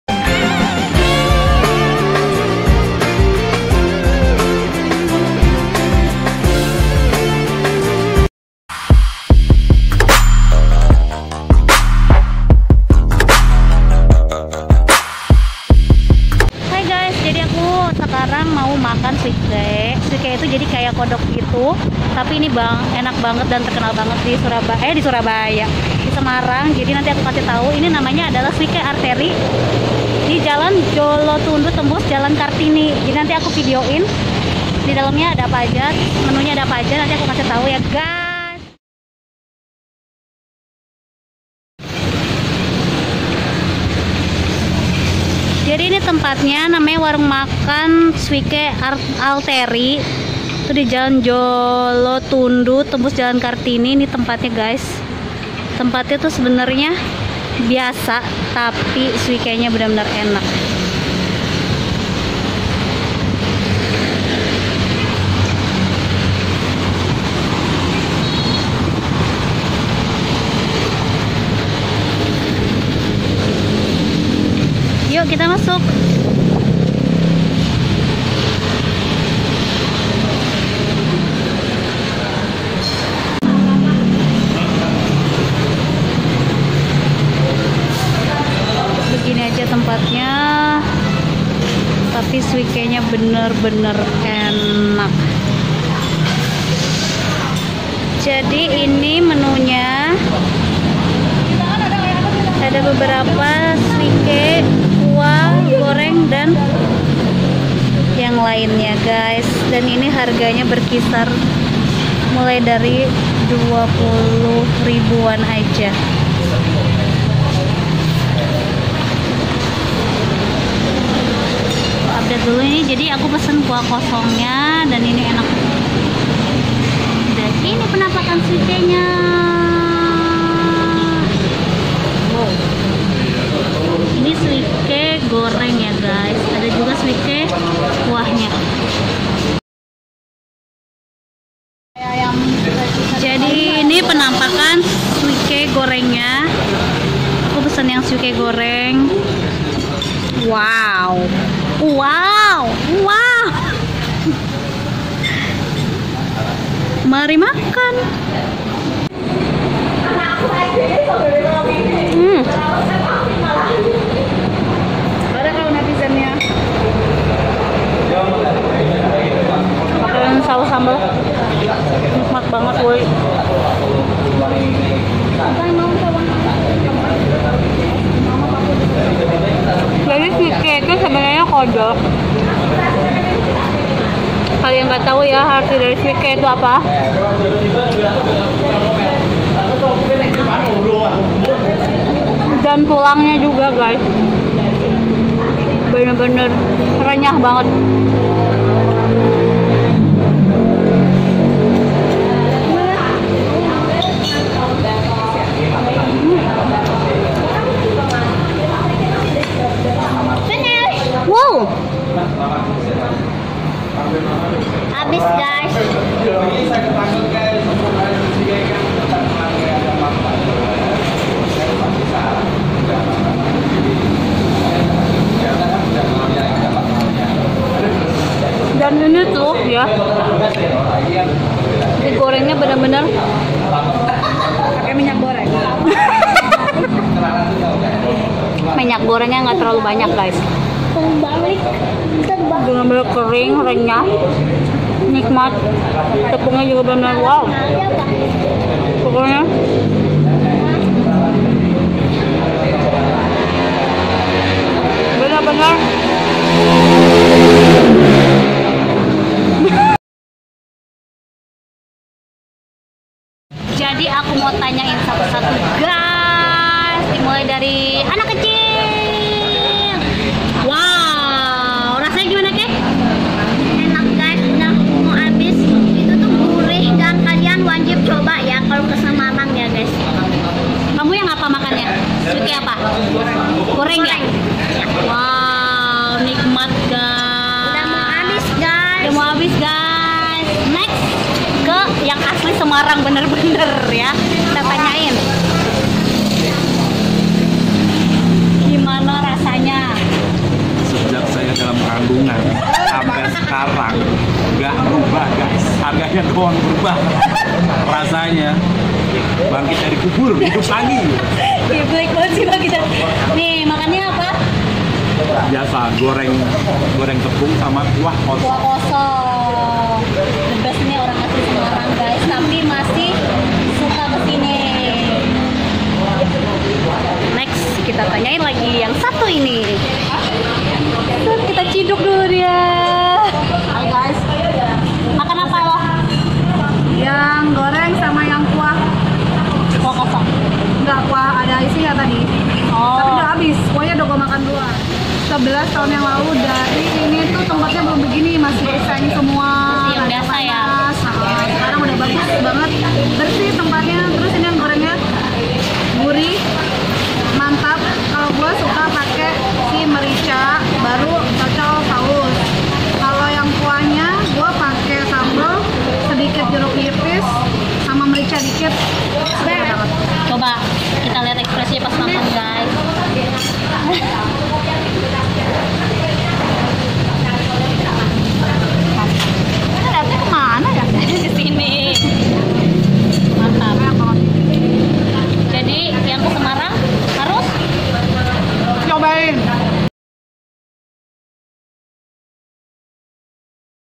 Kuliner Semarang - TEMPAT KULINER SWIKE PARAH ENAKNYA!
Hello Guys kali ini aku kasih tau salah satu tempat yg wajib banget kalian kunjungi kalau ke Semarang ..

Tonton terus videonya sampai habis jangan di skip2 ya videonya 😉😉

Semoga kalian suka sama video ini yaa☺

Selamat Menonton,
Enjoooooy ~~~